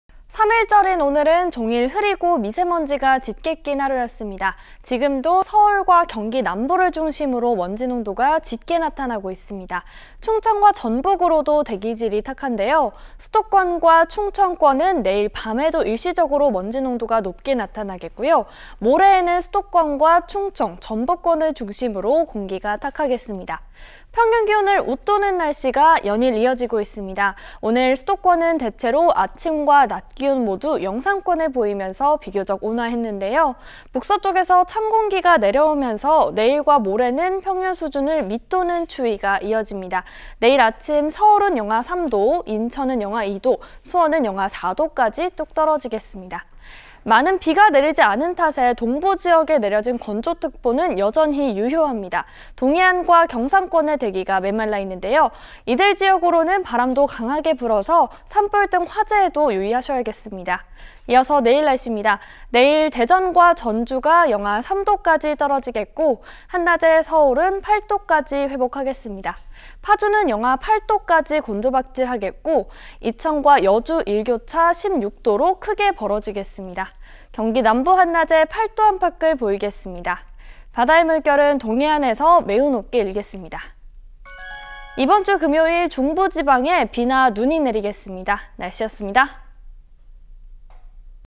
수도권 미세먼지... 내일 다시 강추위
3.1절인 오늘은 종일 흐리고

미세먼지가 짙게 낀 하루였습니다.
지금도 서울과 경기남부를 중심으로
먼지 농도가 짙게 나타나고 있습니다.
충청과 전북으로도 대기 질이 탁한데요.
수도권과 충청권은 내일 밤에도
일시적으로 먼지 농도가 높게
나타나겠고, 모레에는 수도권과
충청, 전북권을 중심으로 종일
공기가 탁하겠습니다.
평년 기온을 웃도는 날씨가 연일
이어지고 있습니다.오늘 수도권은 대체로
아침과 낮 기온 모두
영상권을 보이며 비교적 온화했는데요.
북서쪽에서 찬 공기가 내려오면서
내일과 모레는 평년 수준을 밑도는
추위가 이어집니다.